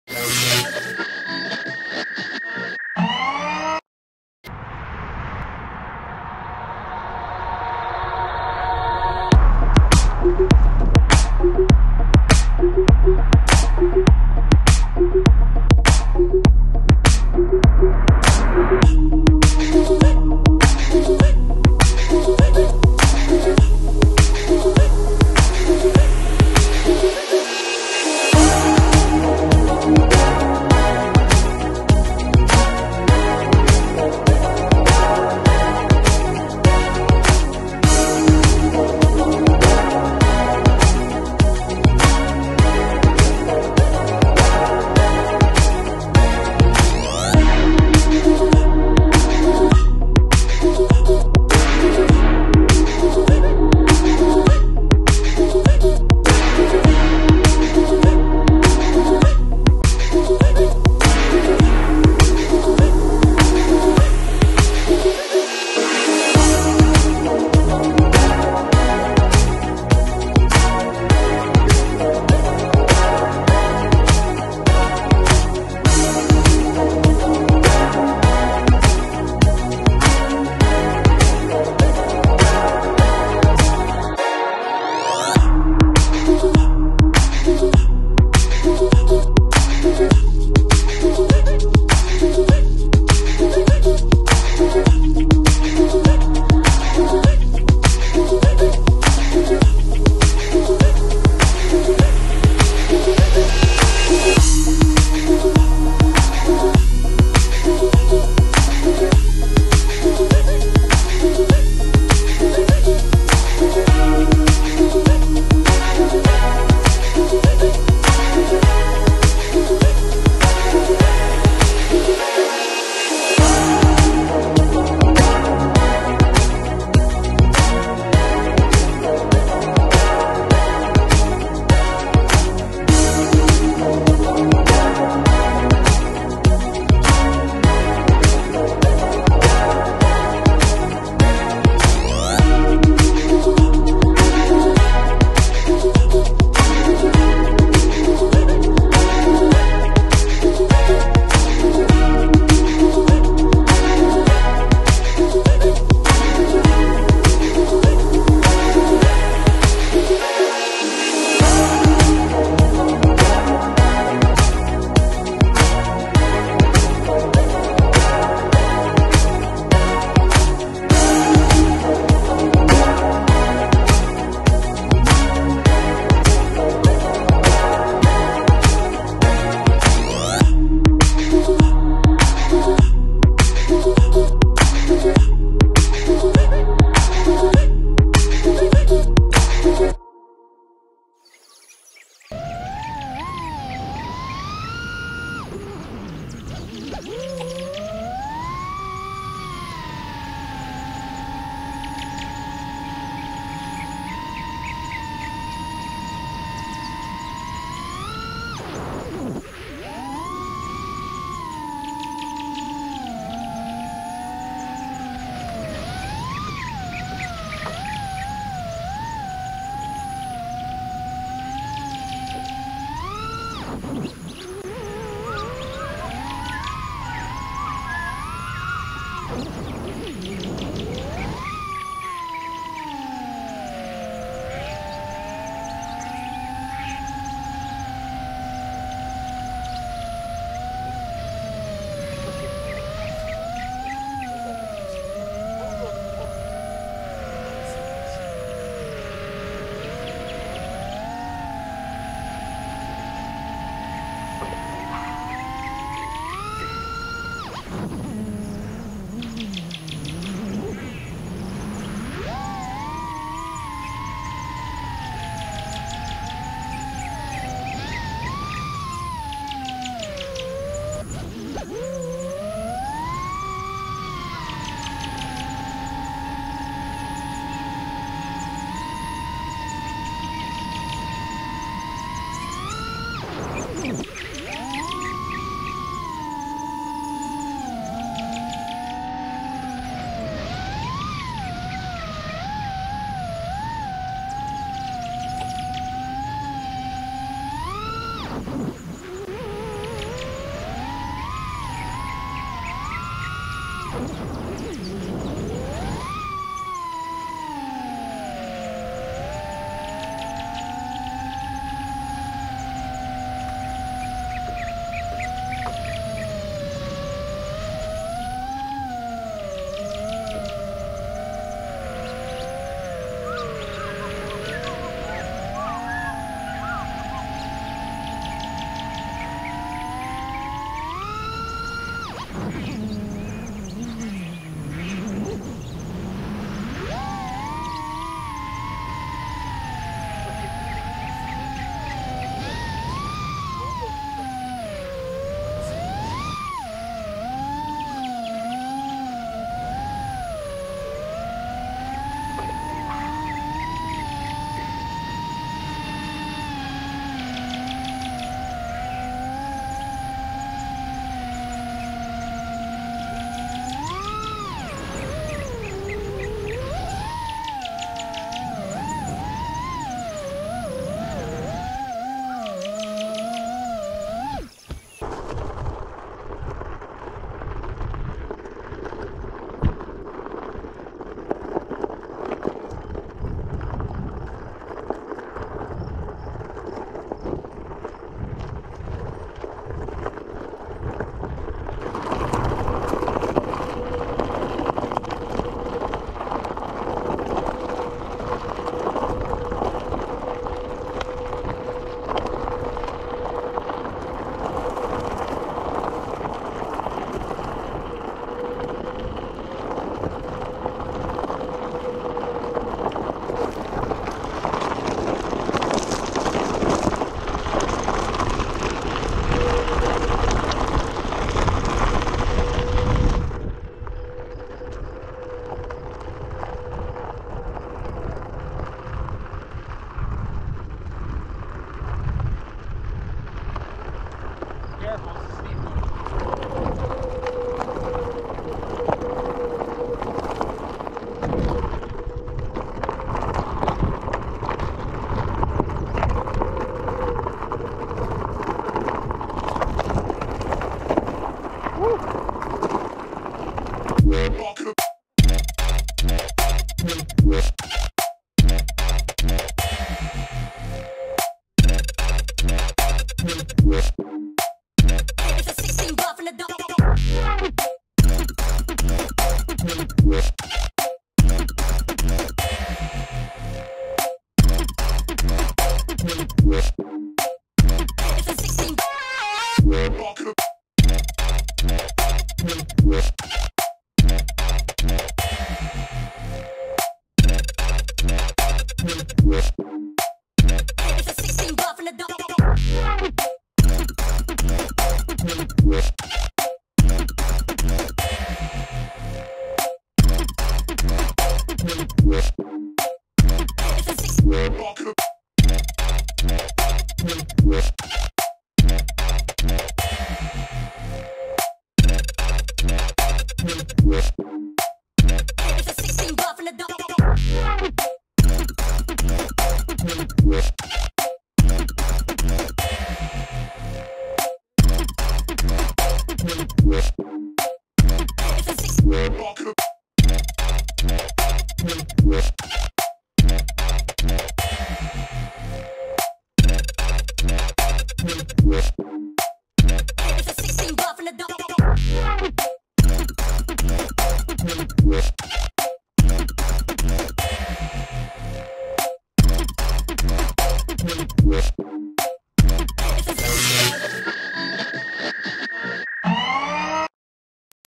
FPV ADVENTURE
FPV adventure on a onewheel gt  with my boy one pack patty. We road up as steep as a onewheel gt could handle. Eventually the gt gave out due to high temperature. Not caught on film but at one point I had to hike with a 30lb FPV bag and a  heavy onewheel gt in my hand. In my opinion worth every step to get this footage. More content coming soon. Stay futuristik my friends.